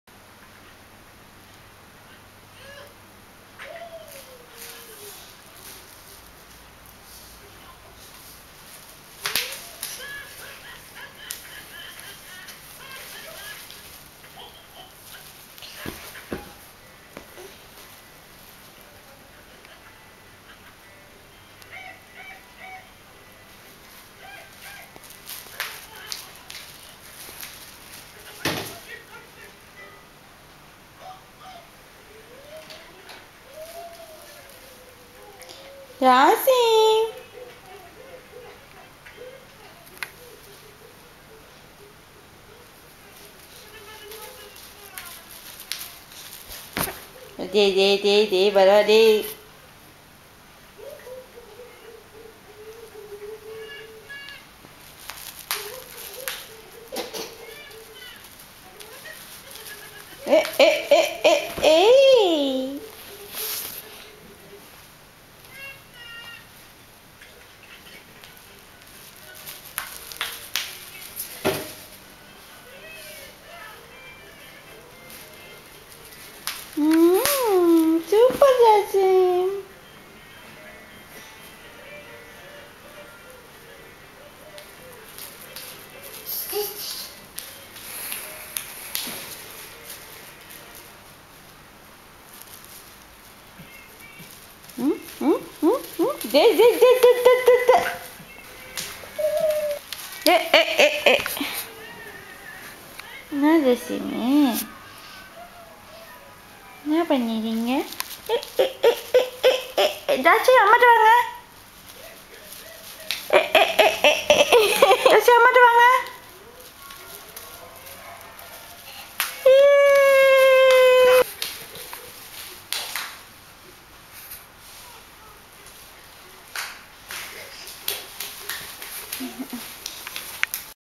Jasim Walking III
Jasim is walking to and fro from sofa to TV